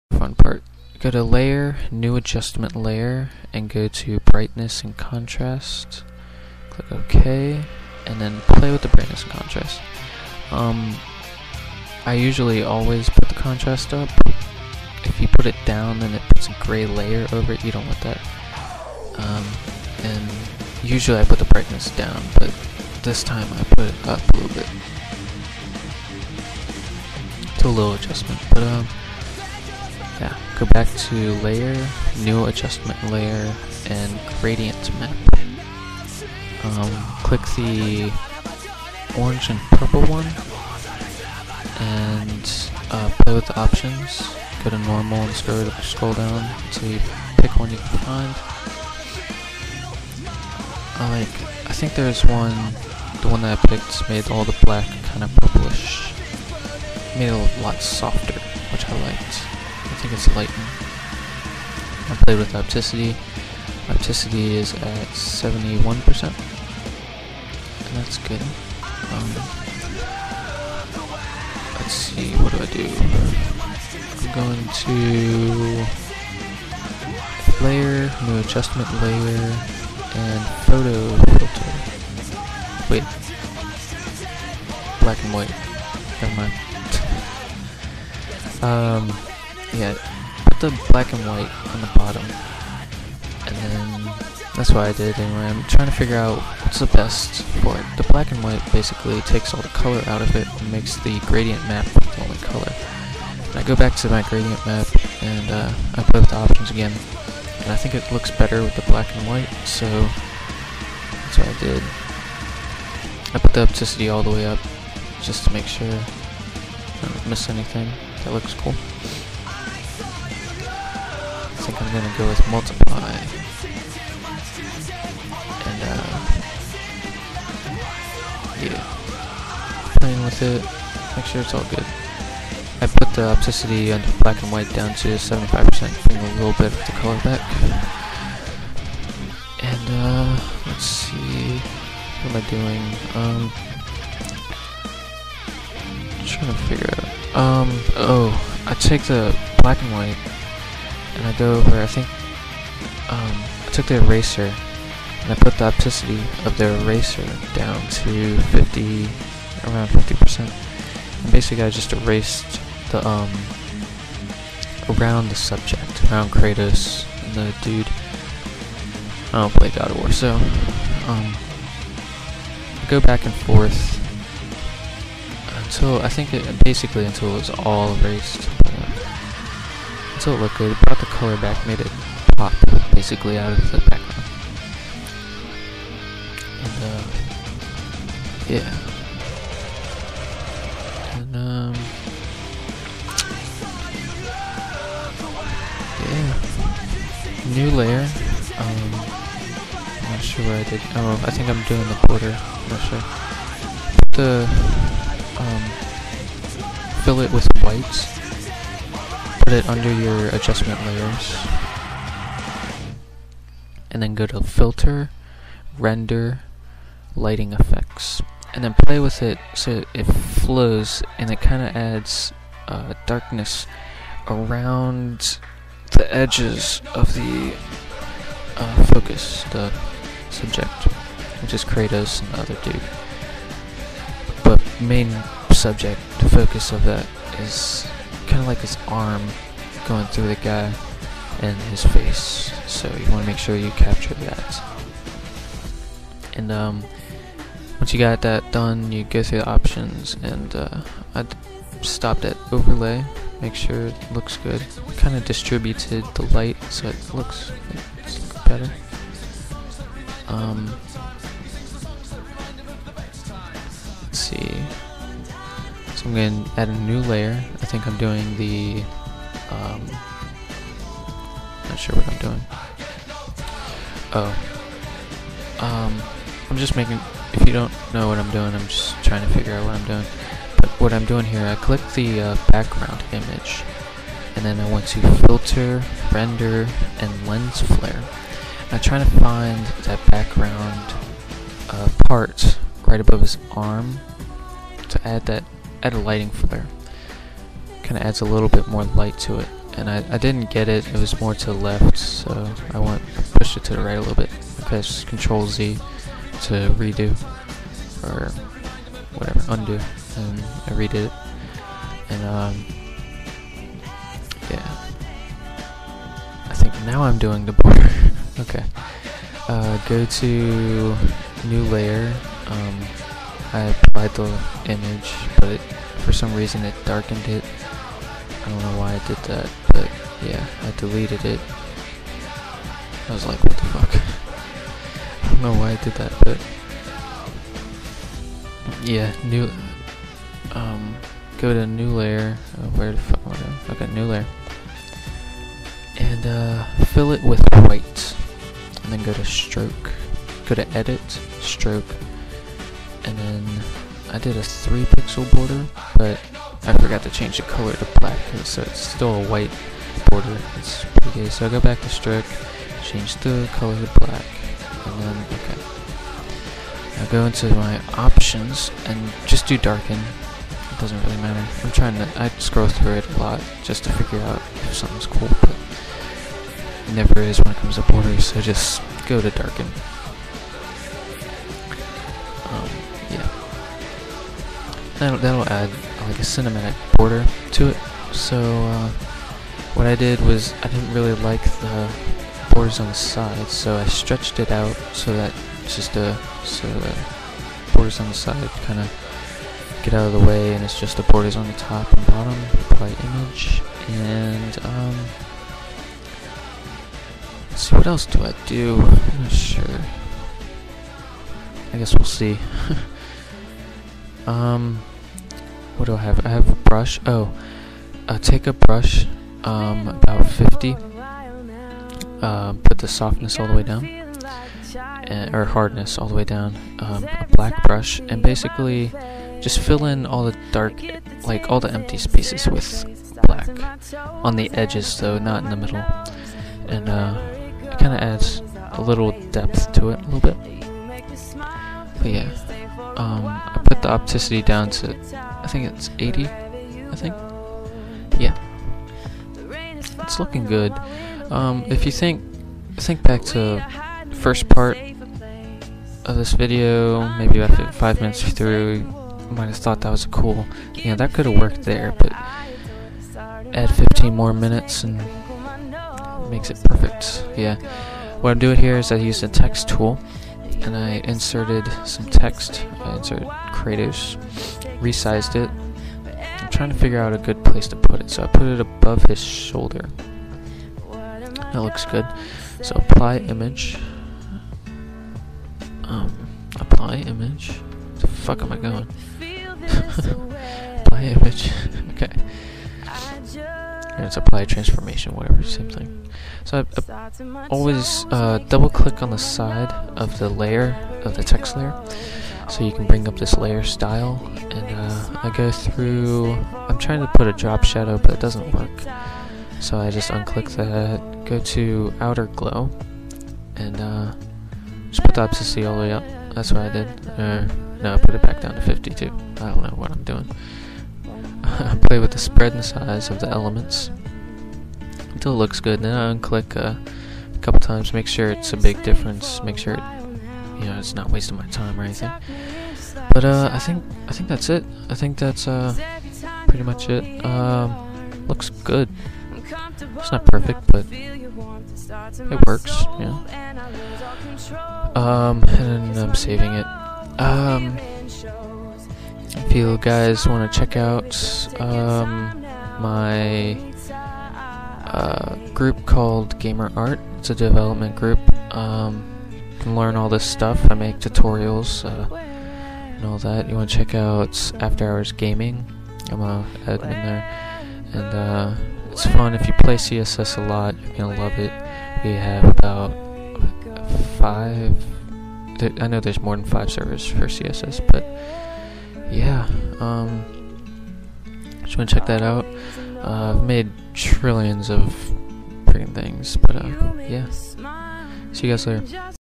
Photoshop CS4 Tutorial - Forum Signature - Kratos p2
Afterhoursgaming.com
This is part one of my Photoshop tutorial for a kratos signature.